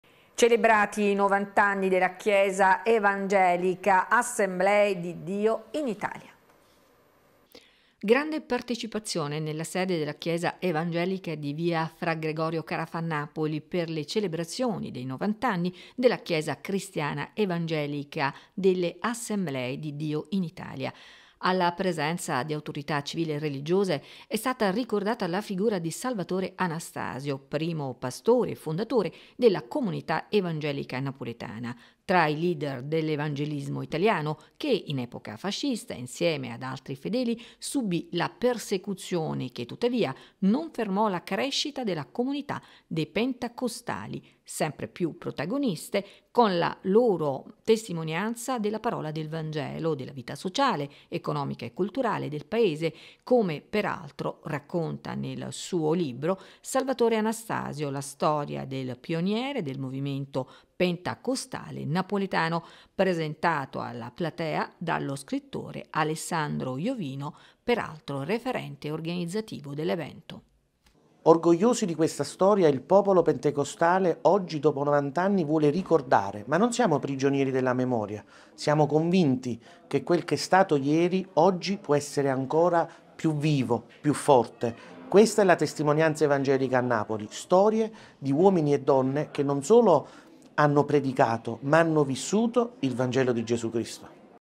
TELEVOMERO - 90 Anni Chiesa ADI di Napoli
DOMENICA 15 GENNAIO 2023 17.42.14

Celebrati 90 anni Chiesa evangelica 'Assemblee di Dio in Italia'

Celebrati 90 anni Chiesa evangelica 'Assemblee di Dio in Italia' A Napoli parterre politico-istituzionale bipartisan per evento (ANSA) - NAPOLI, 15 GEN - Folla, ieri, nella sede della Chiesa evangelica di via Fra Gregorio Carafa a Napoli per le celebrazioni dei 90 anni della Chiesa Cristiana Evangelica delle "Assemblee di Dio in Italia" a Napoli. Alla presenza di autorità civili e religiose è stata ricordata la figura di Salvatore Anastasio primo pastore e fondatore della comunità evangelica napoletana, tra i leader dell'evangelismo italiano, che in epoca fascista, insieme ad altri fedeli subì la persecuzione che, tuttavia, non fermò la crescita della comunità dei Pentecostali sempre più protagoniste, con la loro testimonianza della Parola del Vangelo, della vita sociale, economica e culturale del Paese, come peraltro racconta nel suoi libro "Salvatore Anastasio, la storia del pioniere del movimento pentecostale e napoletano" presentato alla platea dallo scrittore Alessandro Iovino. E di come, da quei tempi ad oggi, Napoli e la Campania siano diventati luoghi di una tolleranza che ha tuttavia ancora bisogno di essere coltivata e praticata, hanno parlato nel loro intervento il presidente della Regione Campania Vincenzo De Luca accompagnato dalla consigliera Paola Raia e il sindaco di Napoli Gaetano Manfredi. Visione condivisa, questa, anche dall'ex sindaco Luigi de Magistris e dall'ex governatore campano ed ex primo cittadino di Napoli Antonio Bassolino presente ieri con la senatrice Annamaria Carloni. Ha partecipato all'evento il senatore Lucio Malan (già componente della commissione parlamentare contro le discriminazioni religiose) presente ieri con i colleghi parlamentari Marta Schifone, Annarita Patriarca e Sergio Rastrelli. Tra le autorità presenti il deputato ambientalista Francesco Emilio Borrelli e l'ex ministro ed attuale sindaco di Benevento Clemente Mastella. Con loro anche i consiglieri regionali campani Severino Nappi , Fulvio Frezza e Pasquale Di Fenza. Una celebrazione, dunque, che ha potuto ripercorrere alla presenza di un parterre politico e istituzionale bipartisan, la storia di una comunità religiosa che dai tempi delle deportazioni e del carcere, con la propria preghiera e con il proprio agire evangelico, ha saputo conquistarsi un importante riconoscimento culturale, sociale e civile. "Siamo commossi - ha affermato il pastore dell'ADI di Napoli Davide Di Iorio -, abbiamo predicato lo stesso messaggio evangelico dei nostri padri: Dio ha ispirato ogni cosa e siamo grati per l'intervento delle autorità". "Sono onorato di aver raccolto il testimone di Salvatore Anastasio e Daniele Melluso - ha concluso - nella guida di questa Chiesa". (ANSA). DLP-COM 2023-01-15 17:39 S44 QBXO CRO